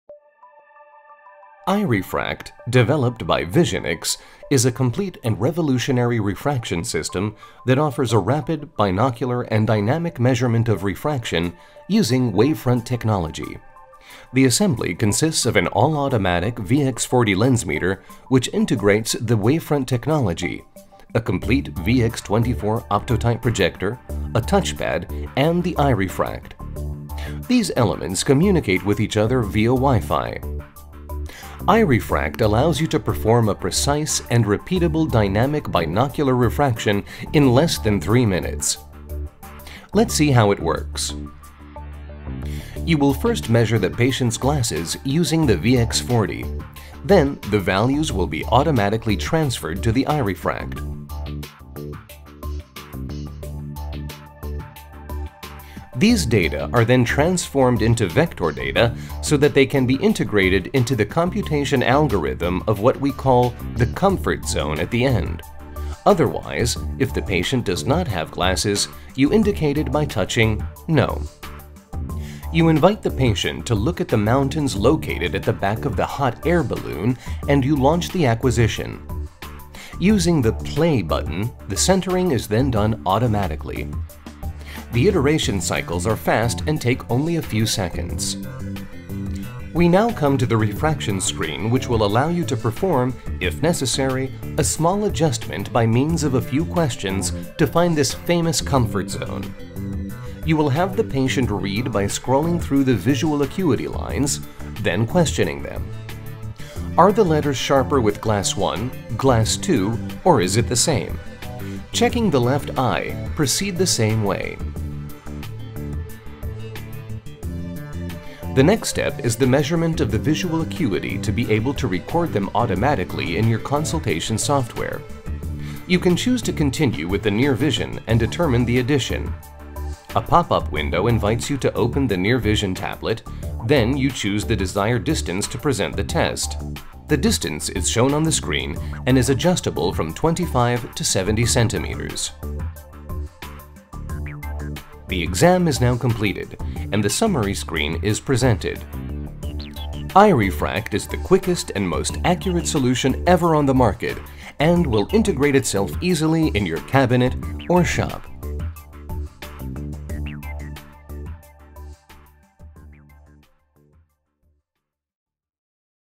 Eye Refract - Dynamic Refraction System
EyeRefract is the first and only dynamic binocular wavefront refraction system that provides consistent reliable refraction results regardless of operator. The typical refraction process usually includes a first reading on an autorefractor followed up by a subjective refraction. With EyeRefract, you can combine these two steps with one instrument to obtain a full refraction, objective autorefraction AND subjective refinement, in about 3 minutes.